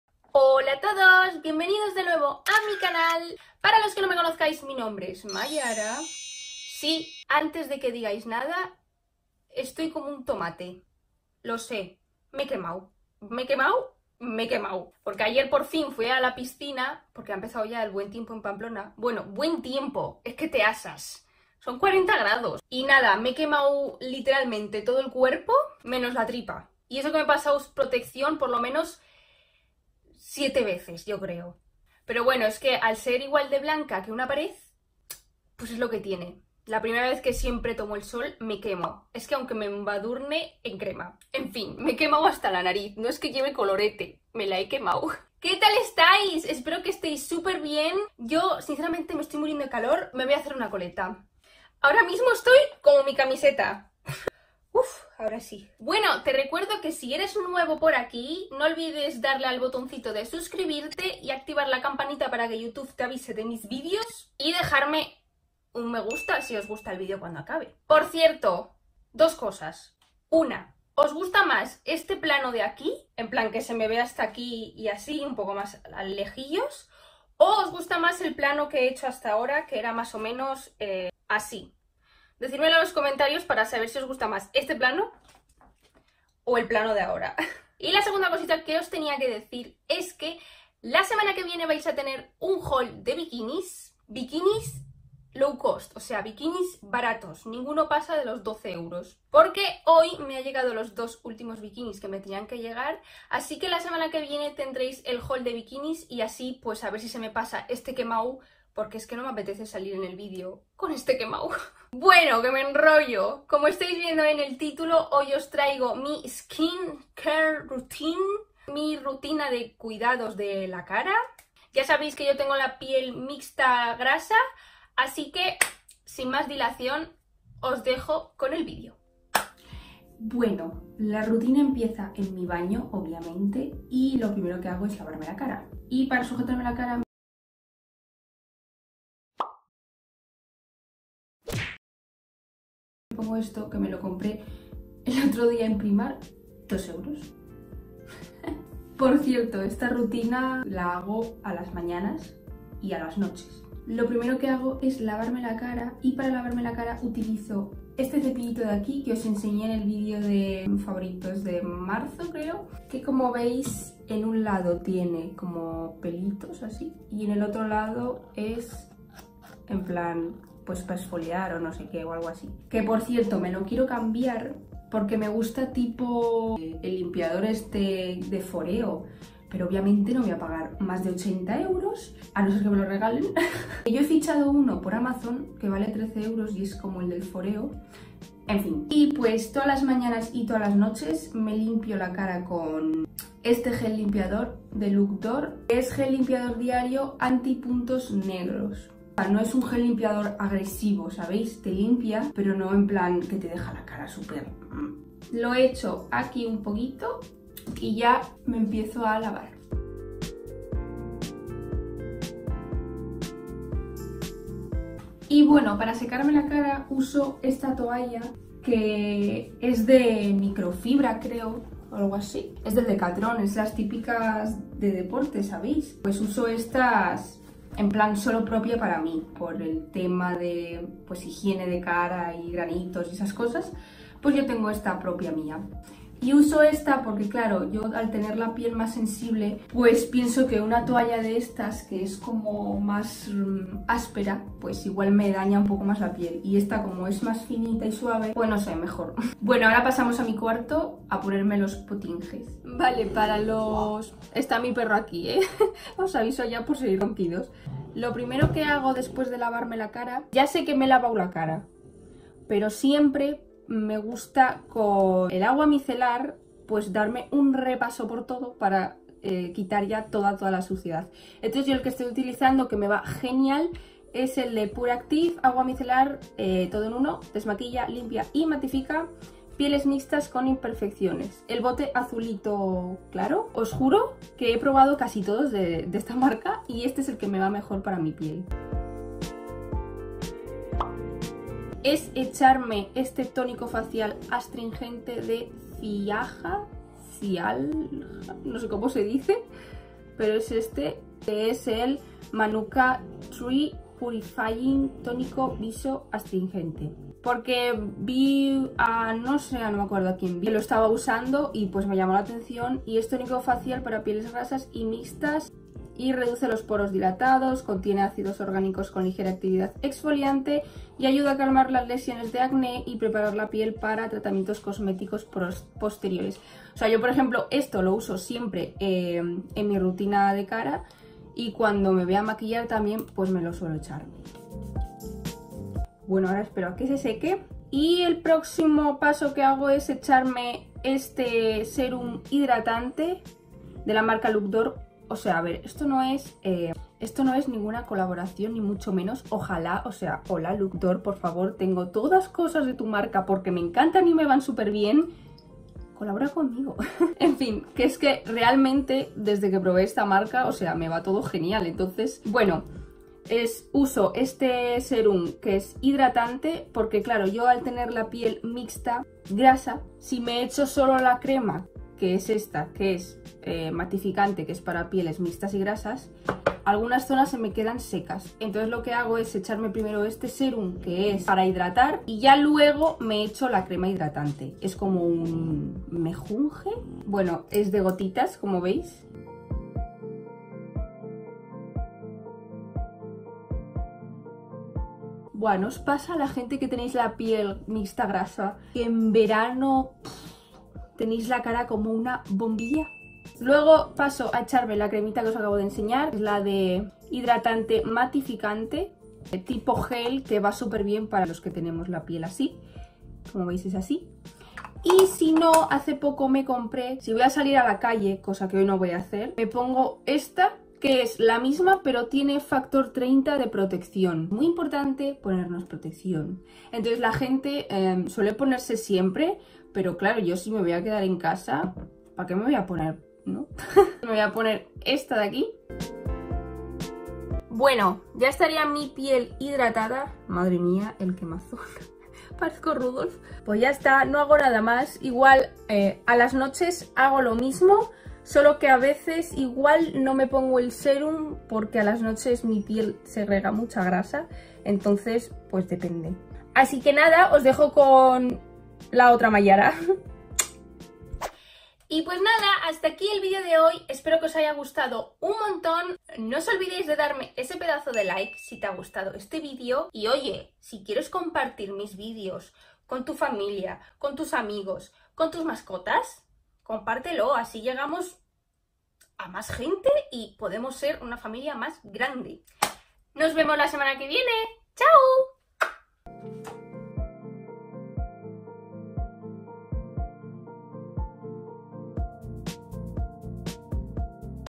MY SKIN CARE ROUTINE 💁🏼‍♀️ | mis cuidados de cara || Mayiyi ♡
Hoy os traigo mi rutina de cuidados de la cara, espero que os guste! 🥰